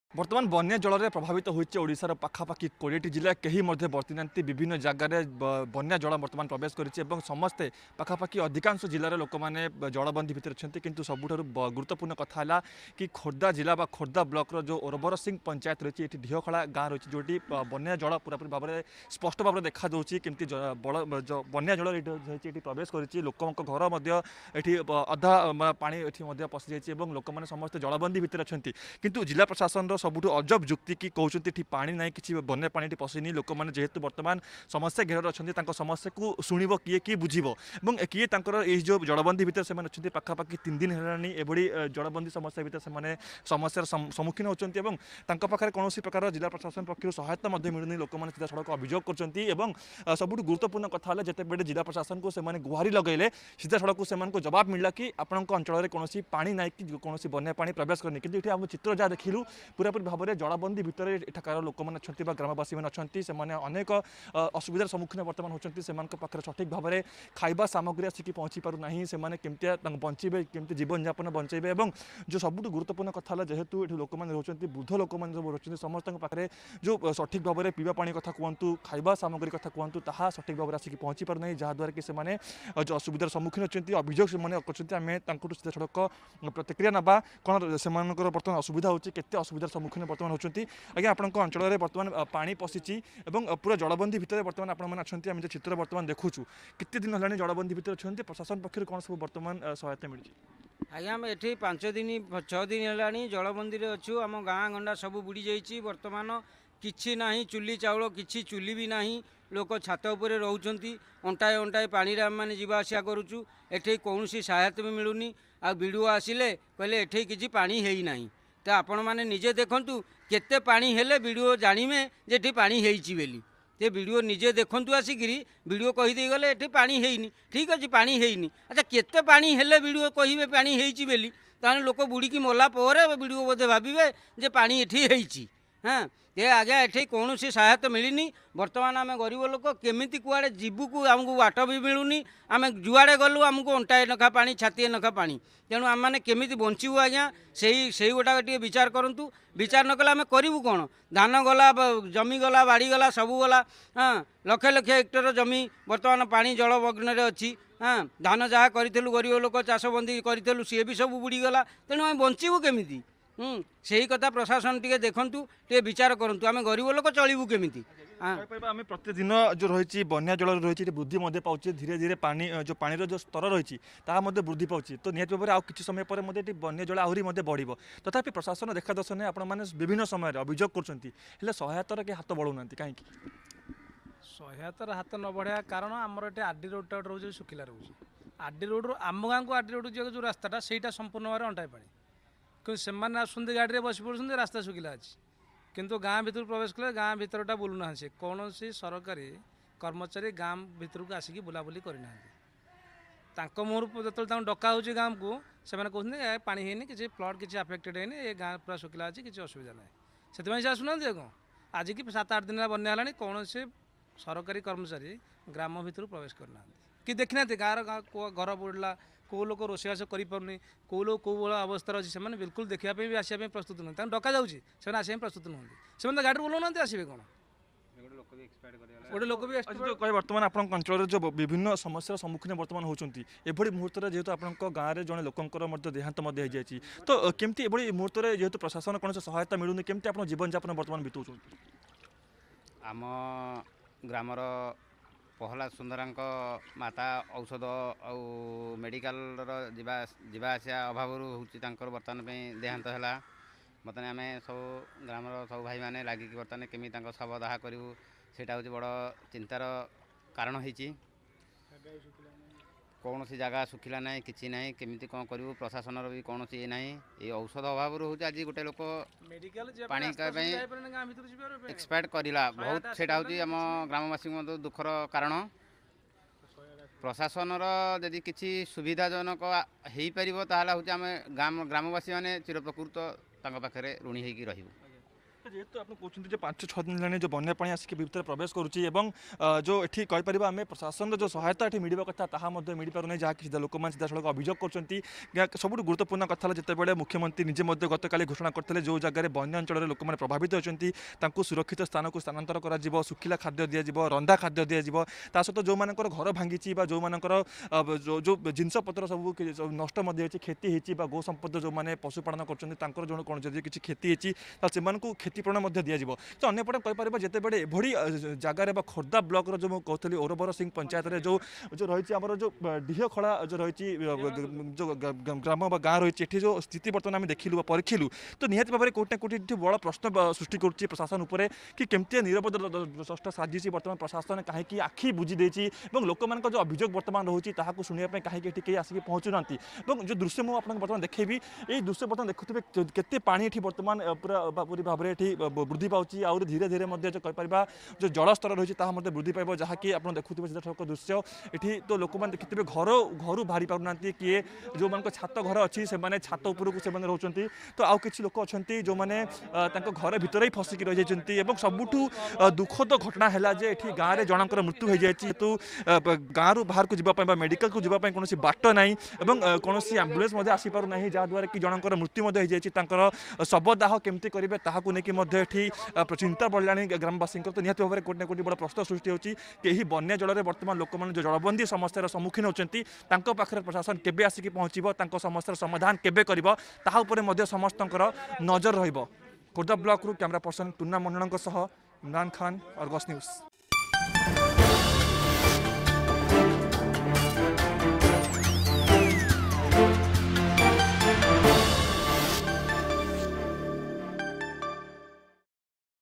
4 ଦିନ ଧରି ବନ୍ୟା ଜଳରେ ବନ୍ଦୀ ଗାଁ ଲୋକେ
4 ଦିନ ଧରି ବନ୍ୟା ଜଳରେ ବନ୍ଦୀ ଖୋର୍ଦ୍ଧା ବ୍ଲକ ଓରବରସିଂ ପଞ୍ଚାୟତ ଡିହଖଳା, ଦୁର୍ଗାପୁର ଗାଁ ଲୋକେ 4 ଦିନ ଧରି ବନ୍ୟା ଜଳ ଭିତରେ ରହିଥିବା ବେଳେ ପ୍ରଶାସନର ଦେଖା ଦର୍ଶନ ନାହିଁ ବୋଲି ଅଭିଯୋଗ ଆଣିଲେ ଗ୍ରାମବାସୀ .